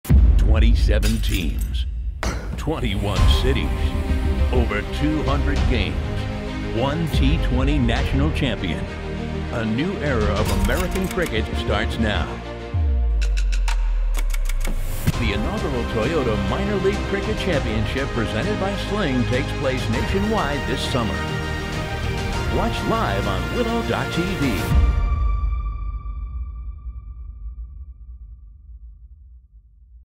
2021 Toyota Minor League Cricket Championship
- The most extensive competition in American cricket history will launch nationwide on July 31, with more than 200 games set to take place in 21 cities.
- Twenty-seven teams will compete for $250,000 in prize money, the largest purse in American cricket

Minor League Cricket (MiLC) today announced a partnership with Toyota to sponsor its inaugural championship season in 2021. The Toyota Minor League Cricket Championship will be presented by SLING TV, with games set to begin on July 31. More than $4,000,000 has been invested by Minor League Cricket and its 27 team owners into staging the tournament, the most extensive competition to ever take place in American cricket.